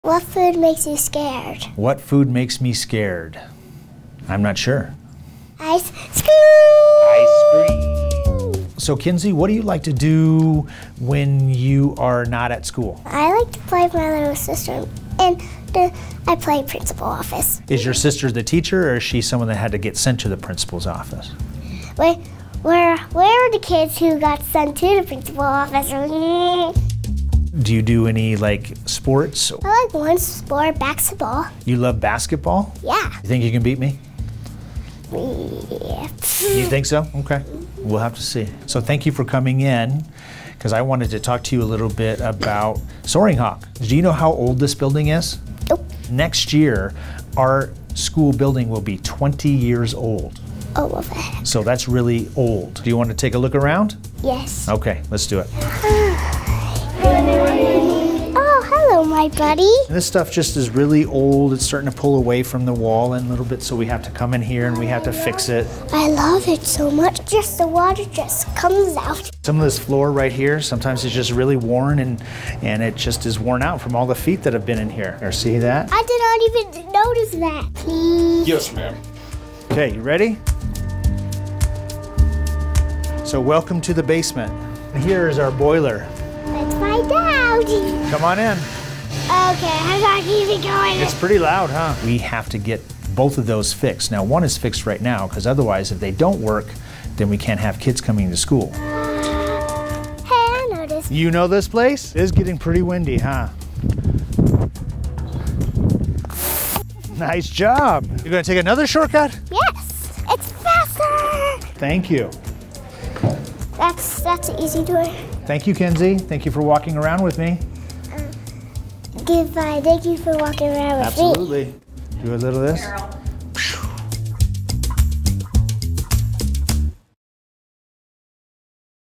Kids with Questions: Kenzie drops in to the office to visit Soaring Hawk Principal Stacey Roberson
She ends up in the principal's office and has some tough questions for her principal, Soaring Hawk's Stacey Roberson. Join Kenzie on a tour of her school.

Let's be ready for Kenzie!